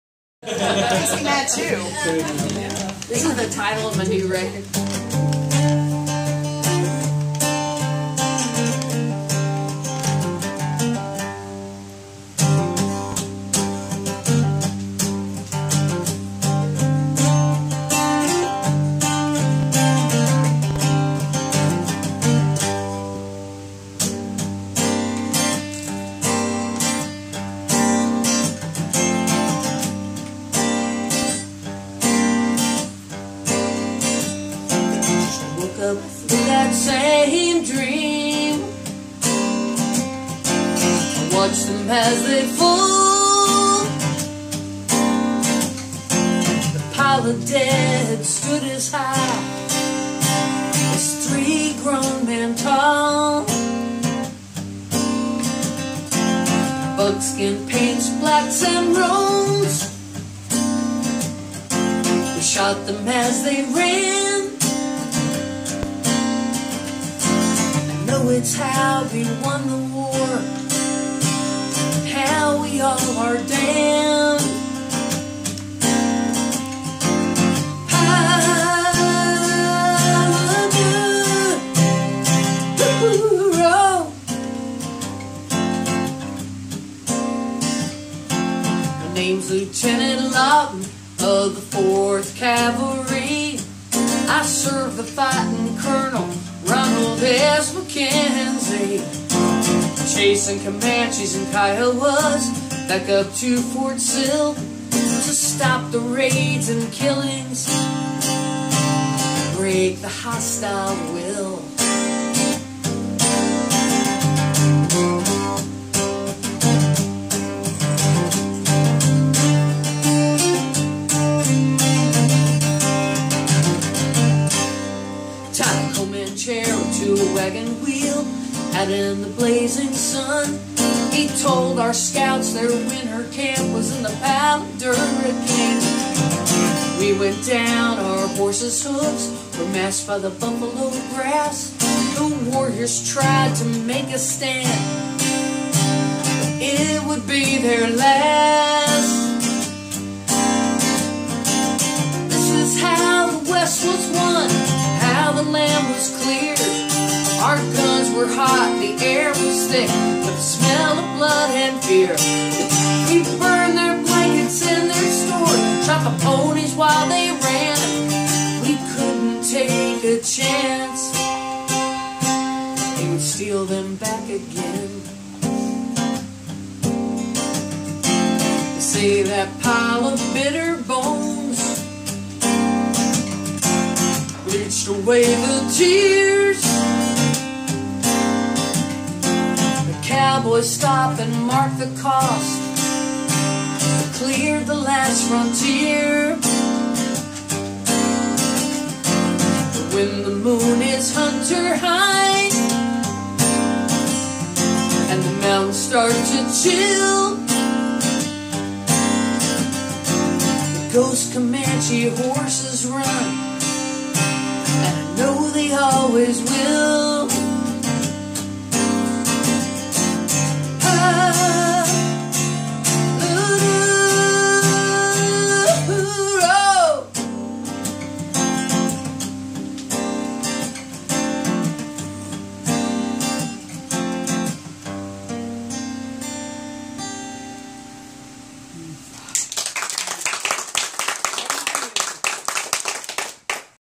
Kiya Heartwood of Stealin' Horses 2016 , Duluth, GA House Concert
Kiya Heartwood is an award winning singer-songwriter who has just returned from touring her one woman show, Song Tales from the American Edge in Ireland and the UK.

The UK's Broadway Baby describes Kiya Heartwood (Stealin Horses, Wishing Chair) as "an award-winning American singer-songwriter who writes smart, funny and poignant songs about the famous and not-so-famous legends of America."